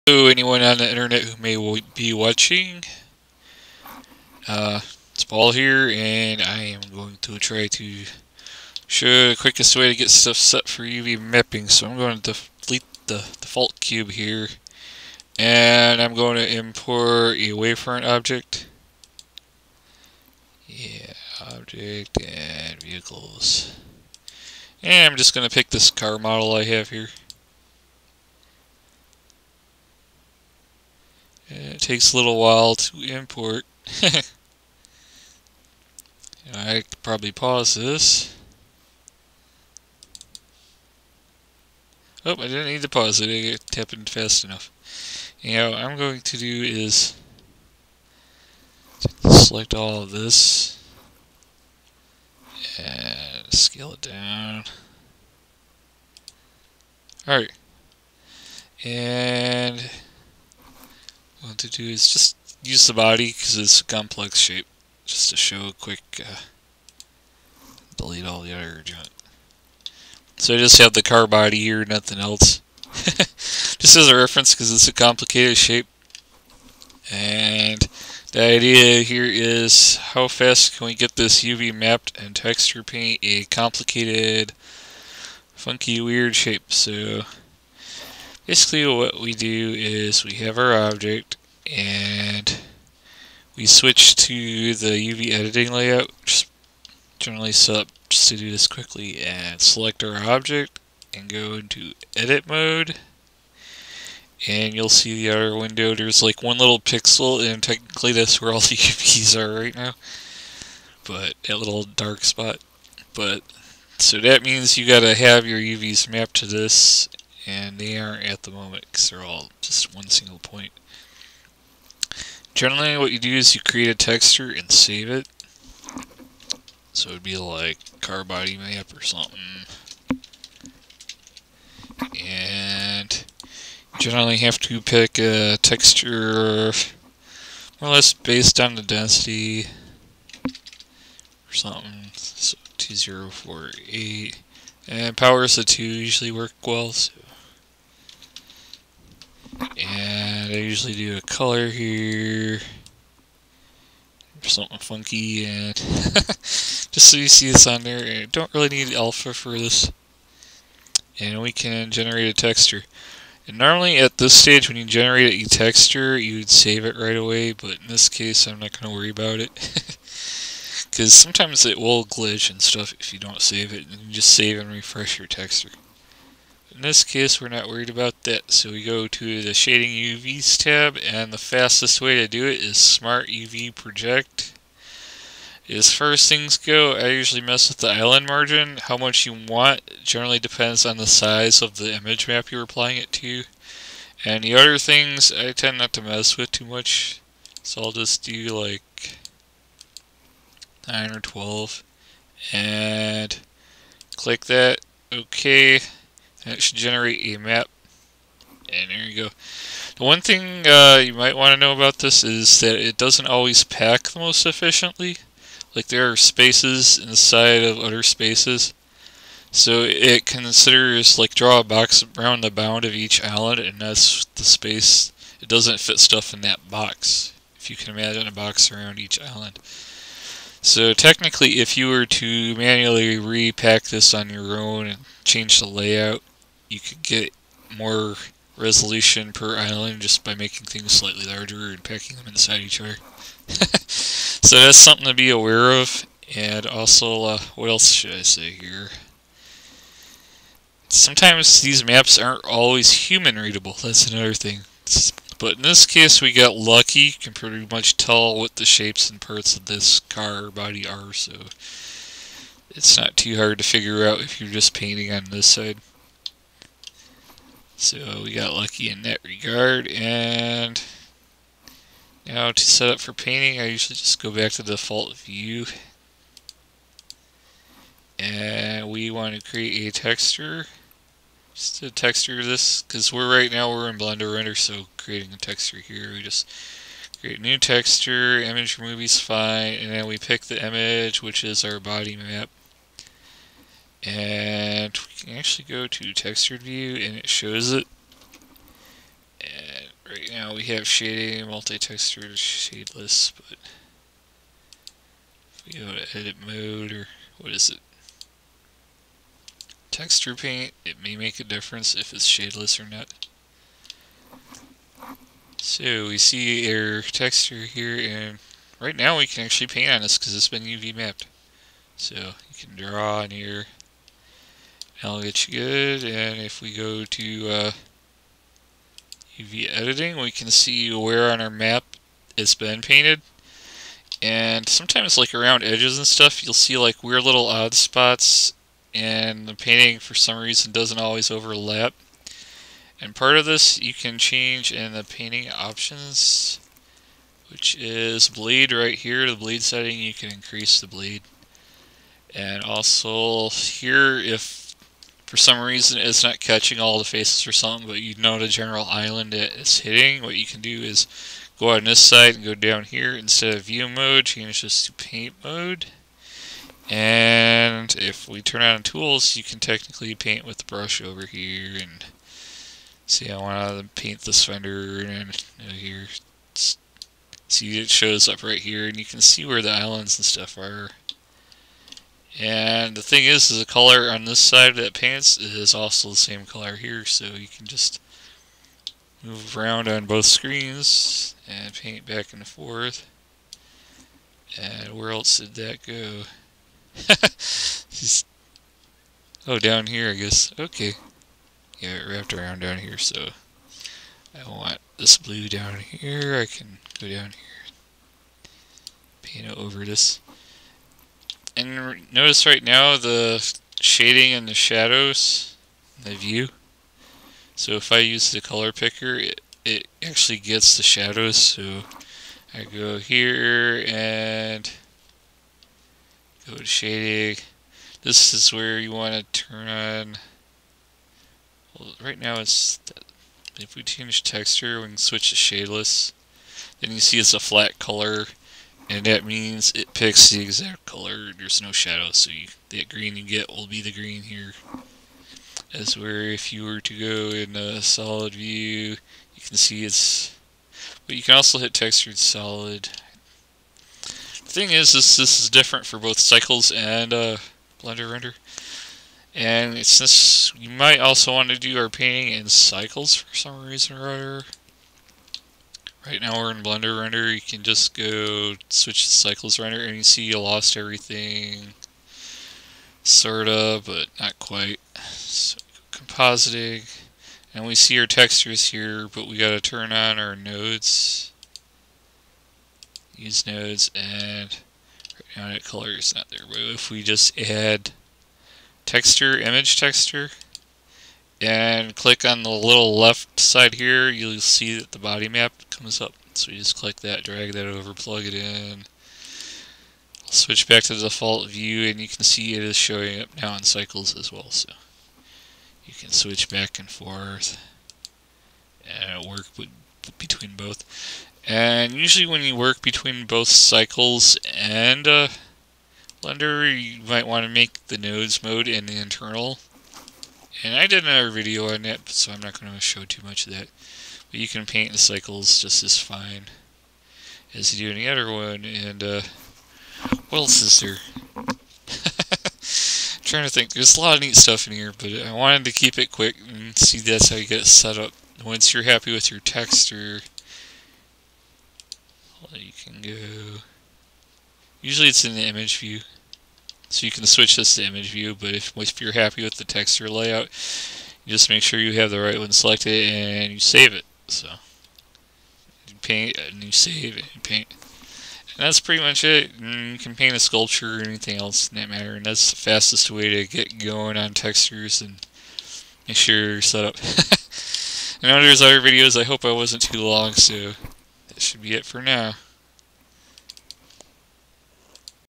Quick Texture Paint Setup in Blender (v.2.74)
A basic and fairly quick setup for UV'ing and texture painting in Blender. This was using 2.74, so I'm not sure how much older this workflow would work.